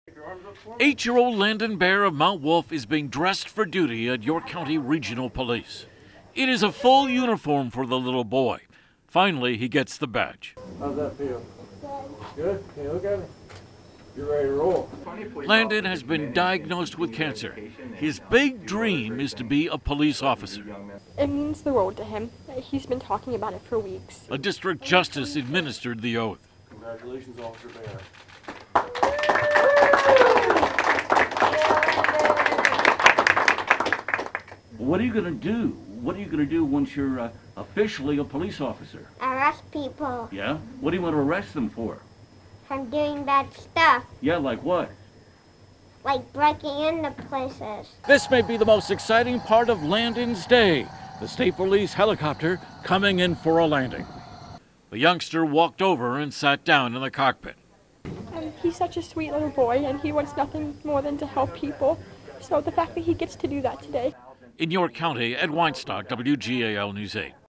York County boy's wish to become police officer comes true
An 8-year-old York County boy's wish came true Friday morning thanks to Make-A-Wish.